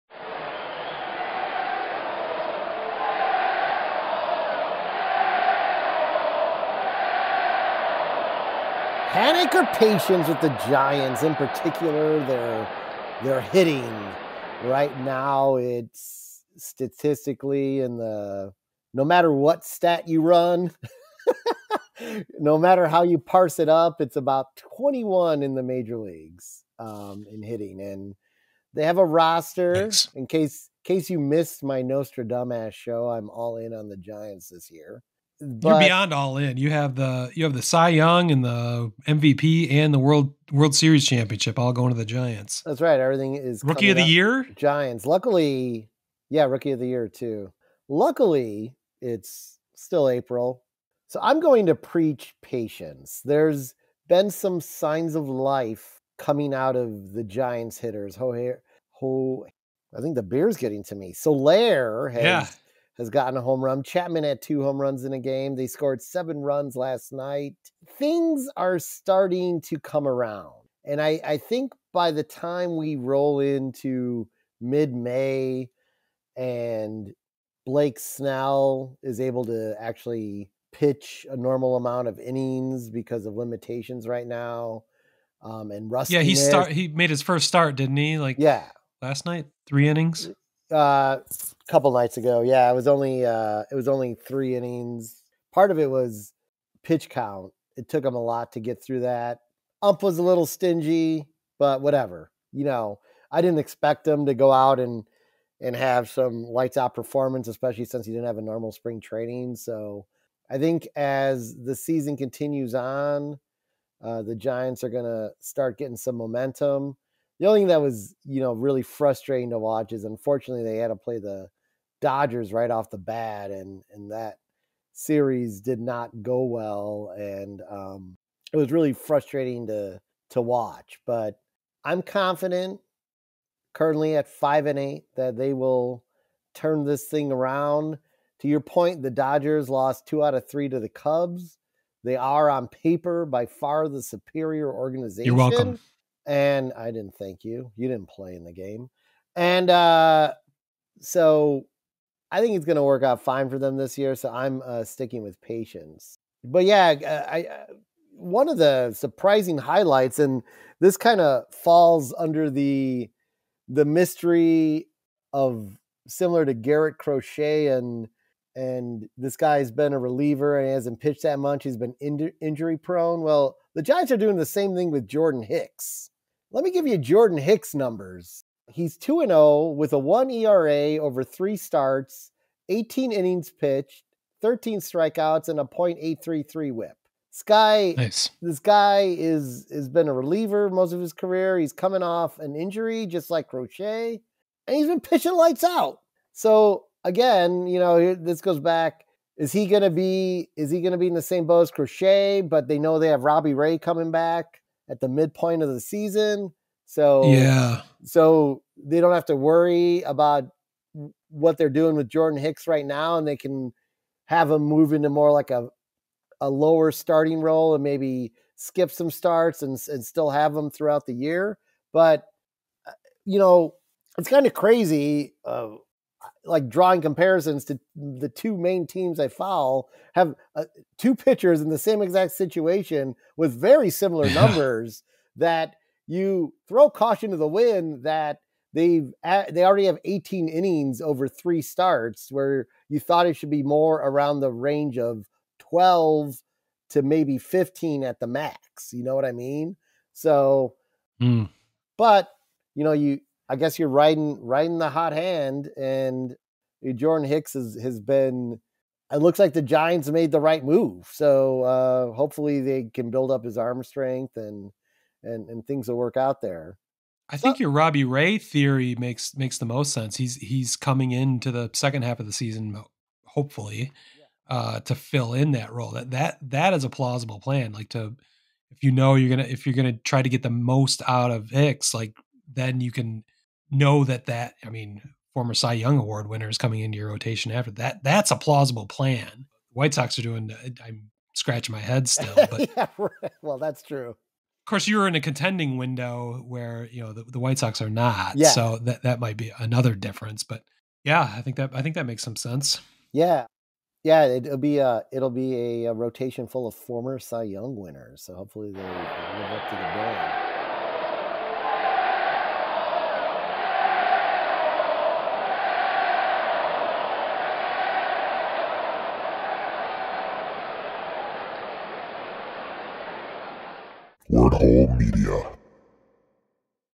Time for Panic or Patience in SF?
How concerned should fans be as SFGiants get off to a disappointingly slow start in arguably the toughest division in baseball?